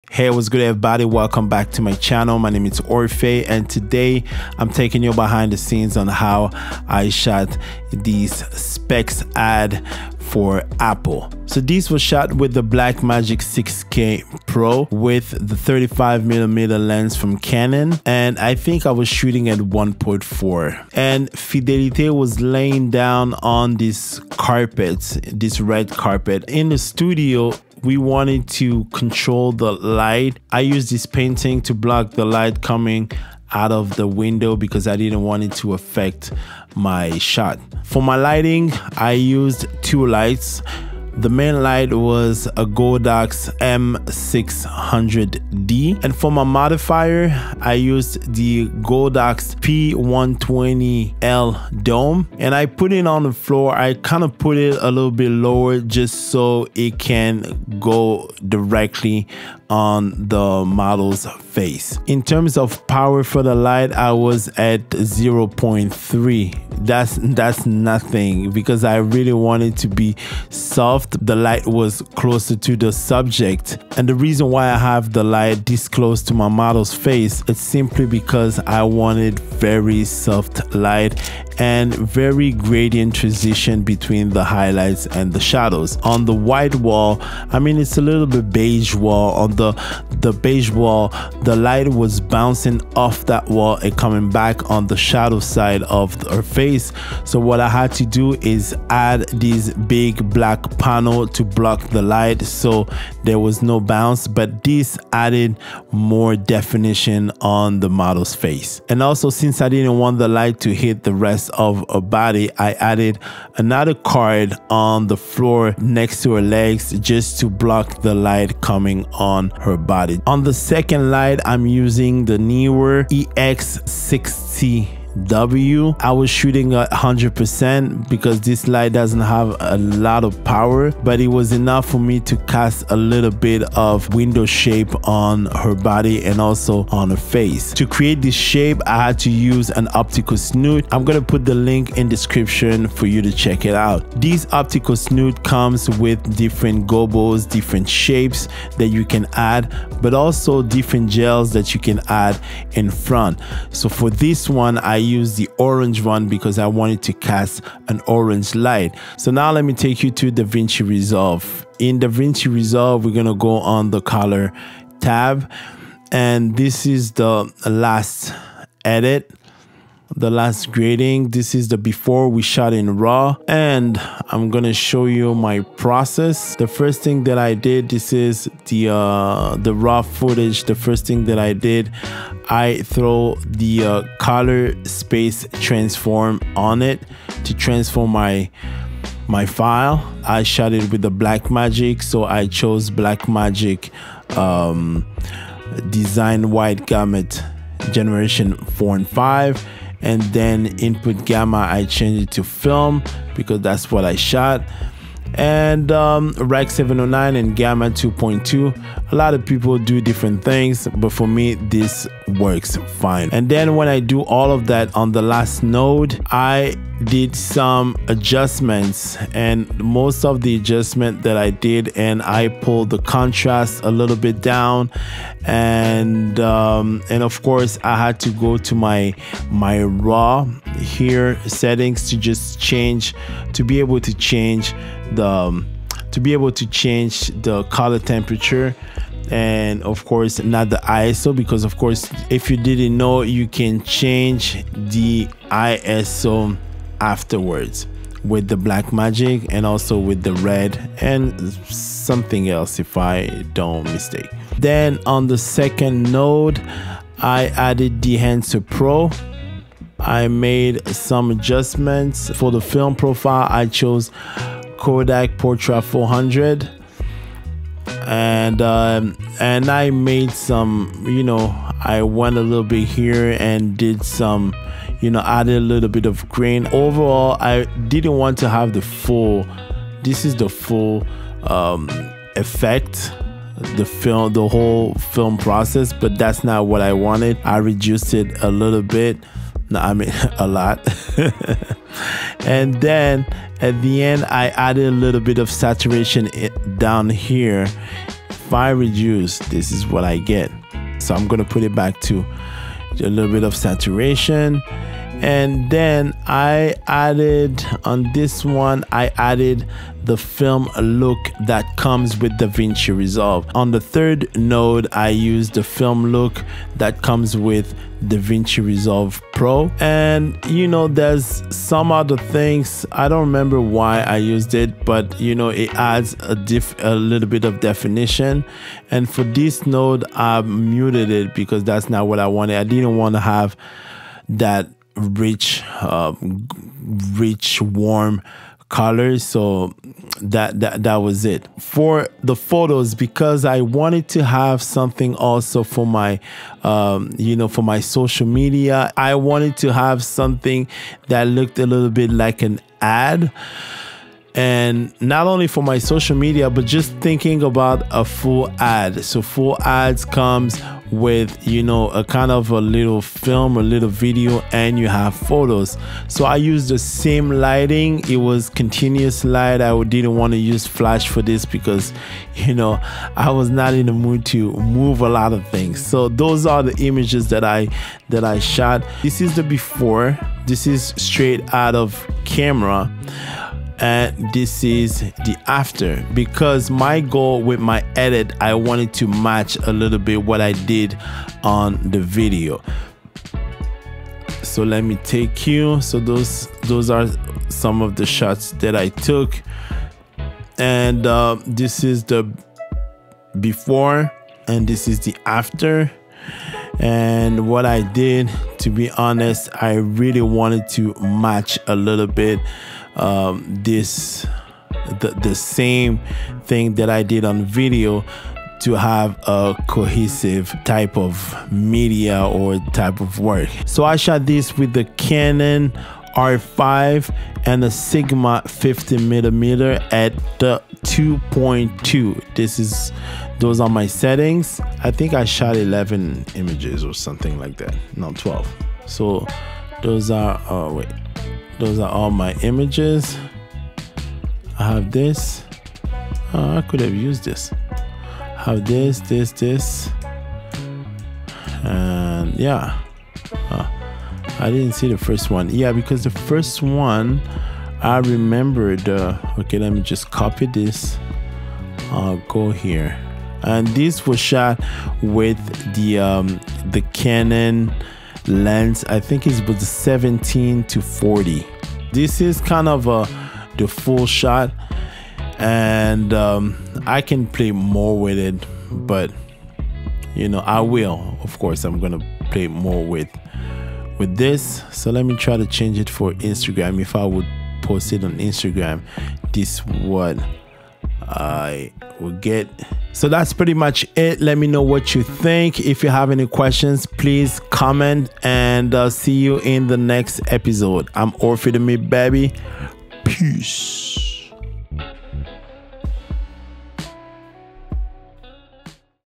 How I Shot an Apple-Inspired Specs Ad: BTS, Lighting Setup, Blackmagic & Canon R5
Join me behind the scenes as I walk you through the process of shooting an Apple-inspired specs ad. In this episode, I’ll show you my lighting setup, the camera gear I used, and how I captured both video and photos for this project.

Highlights of this video include:

Lighting techniques
How I used the Blackmagic 6K Pro for video and the Canon R5 for photos
The role of the optical snoot in adding depth and detail to the lighting
Tips for shooting high-quality specs ads in both photo and video

Gear used:
Optical Snoot
Godox P120L
Godox Light

00:00 Intro
00:10 BTS
02:38 Color Grading Davinci Resolve
05:41 Capture One Installation
08:12 Outro